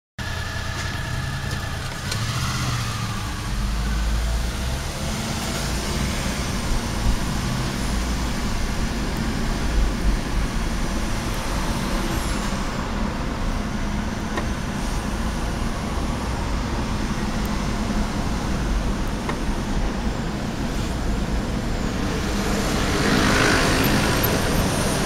白バイが隣りに来たので（信号待ち）・・・眺める
神奈川1号横羽線下の産業道路で、白バイが隣りに来たので（信号待ち）発車・・・右折車線に
行く様子。（天気が良いので、今日は白バイを良く見るなと思う）
2012年5月20日